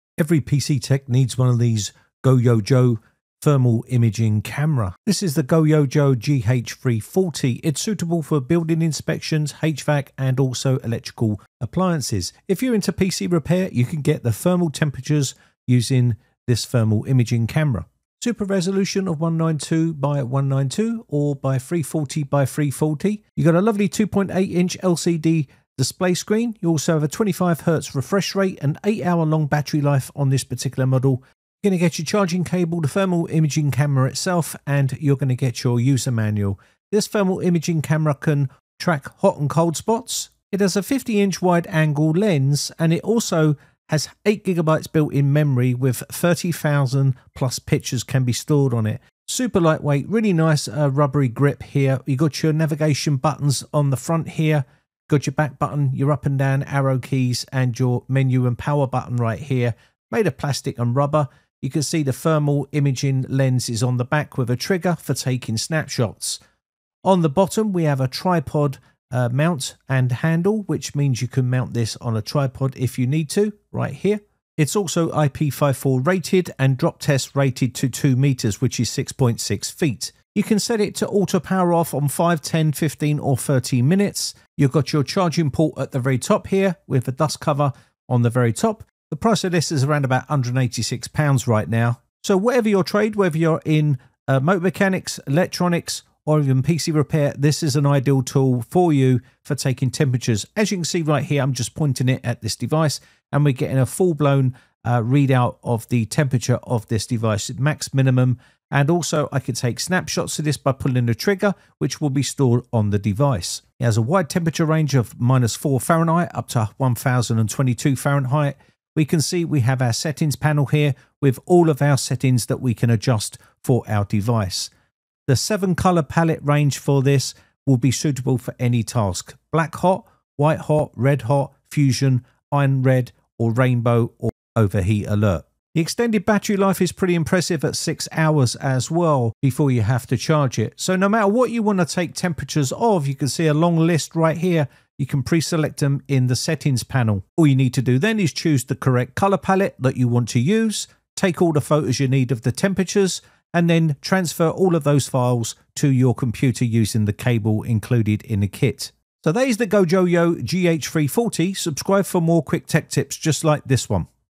Every PC Tech Needs One Of These
GOYOJO GH Series is suitable for building inspection, HVAC, and electrical applications.
Thermal imaging, fusion, and visible light modes can meet diverse application requirements across various scenarios
Choose Between a 256x192 or 96x96 IR Sensor, both with 25 Hz Refresh Rate
2.8" LCD Display with 240 x 320 Resolution
50° Wide Angle Lens
–20°C to 550°C (-4°F to 1022°F) Temperature Range
Precise Readings with ±2% Accuracy
Auto Hot/Cold Spot Tracking
Thermal imaging/Fusion/Visible light
8GB Built-in Memory for 30,000+ Pictures
IP54 Rated & Drop Tested to 2 m (6.6 ft)
6-8 Hours Runtime on a Single Charge
Auto Power-Off  5/10/15/30mins